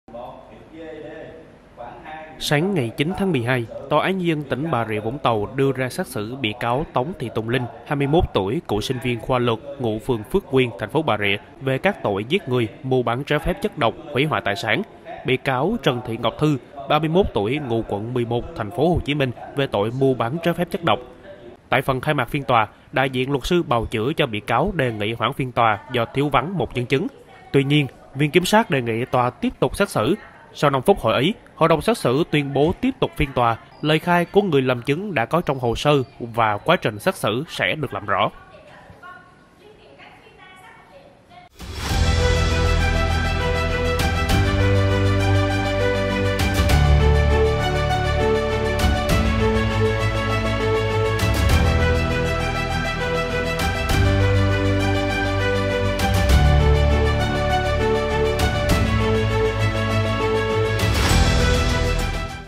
Đang xét xử nữ sinh viên giết cha bằng xyanua - PLO
Theo cáo buộc, do mâu thuẫn trong quá trình sinh hoạt hàng ngày, Tống Thị Tùng Linh nảy sinh ý định giết chết cha ruột mình là ông Tống Hồng Điệp.